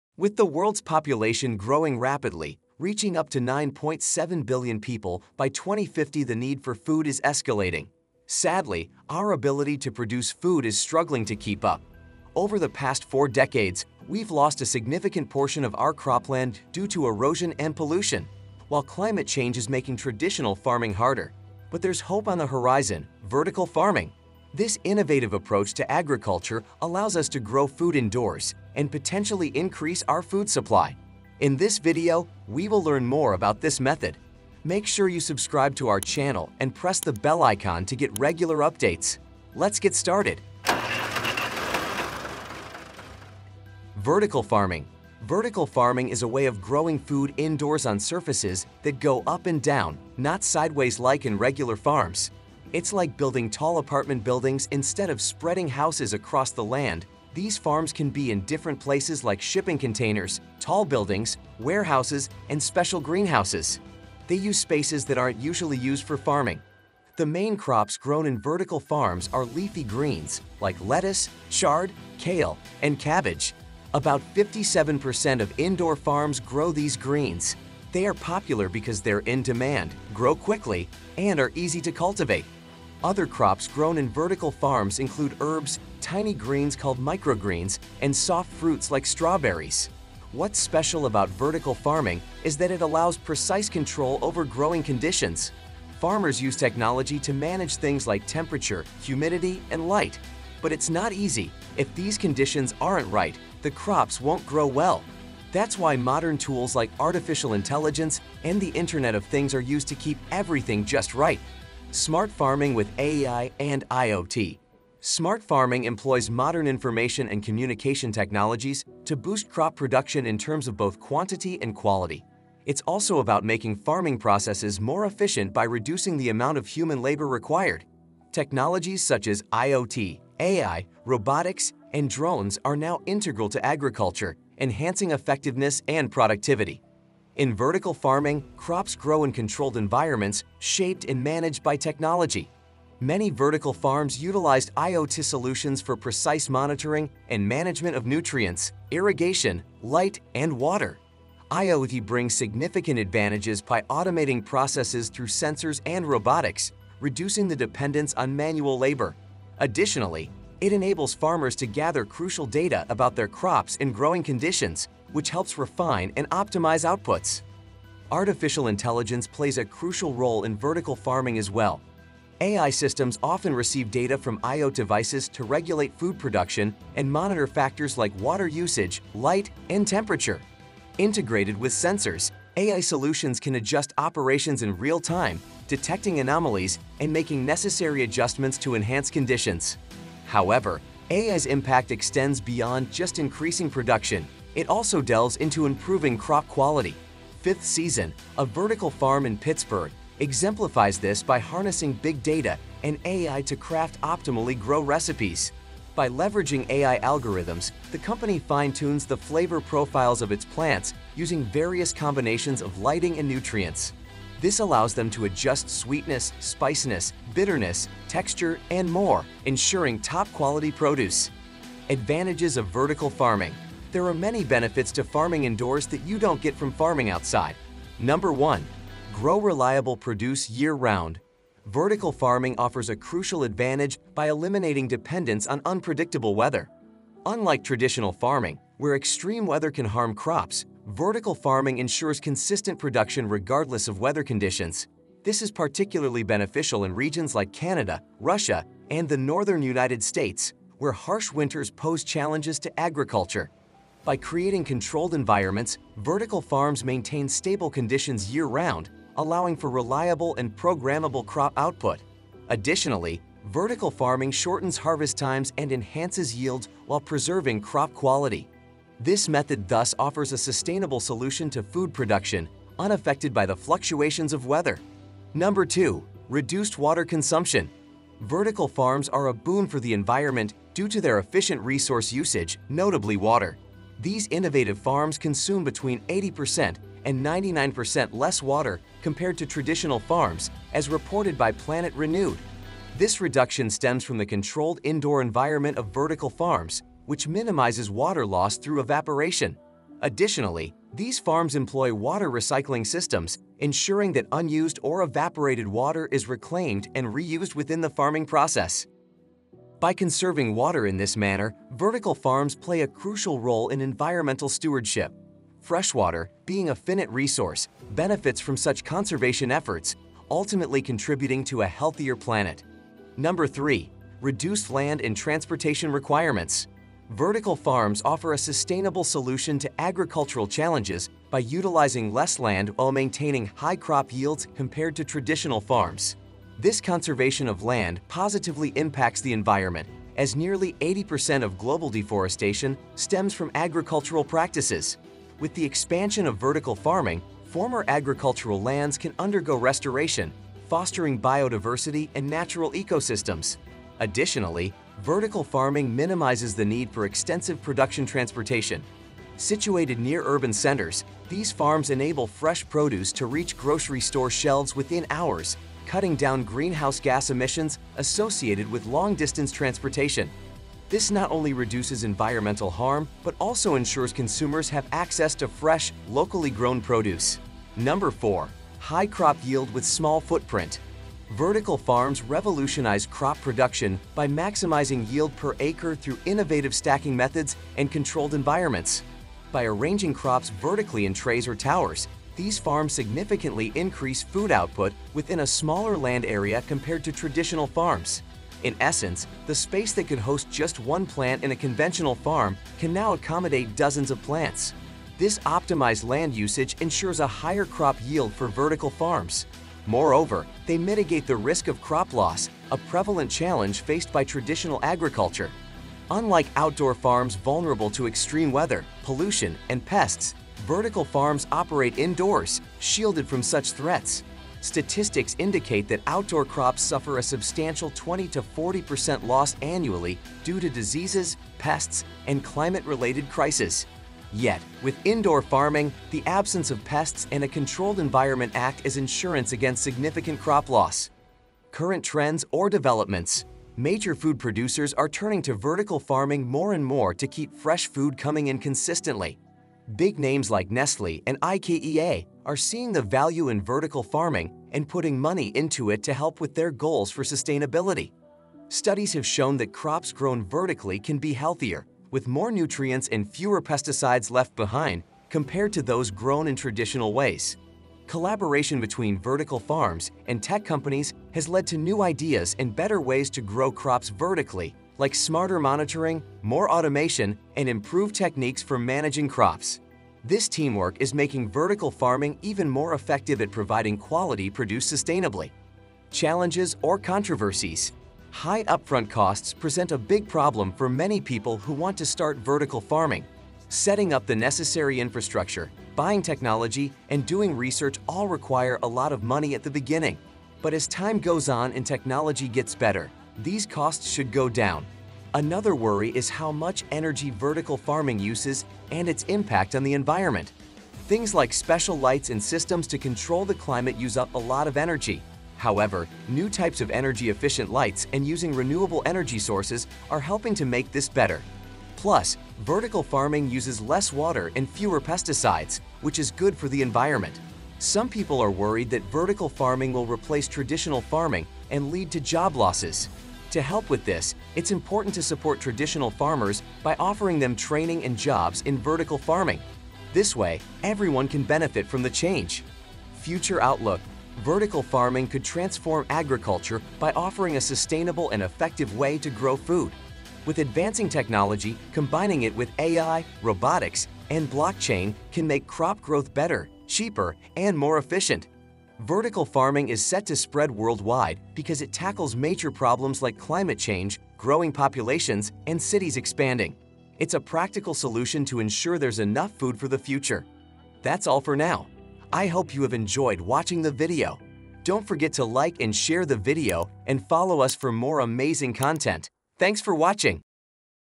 You Won't Believe How Farming In Clouds Looks Like
Discover the future of crop cultivation in our latest YouTube video on vertical farming! 🌱 Dive into the world of innovative agricultural practices that maximize space and resources while minimizing environmental impact. Join us as we explore the heights of vertical farming technology and its potential to revolutionize food production worldwide. Don't miss out on this exciting glimpse into the future of farming!